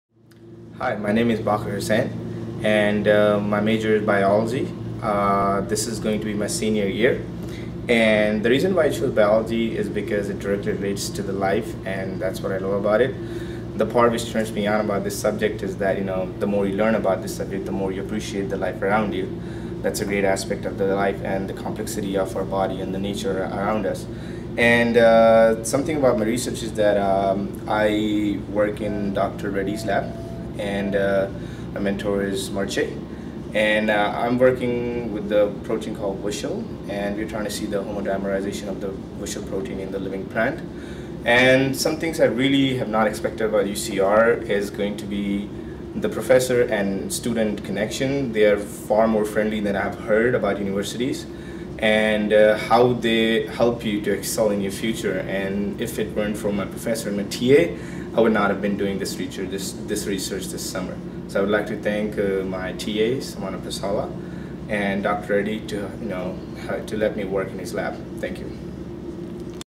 Undergradute Biology Student Baqir Husain
Baqir is a biology undergraduate student doing research in Botany & Plant Sciences in  Dr. Gonehal's lab at UC Riverside. His research objective is to demonstrate WUSCHEL homodimerization in vivo.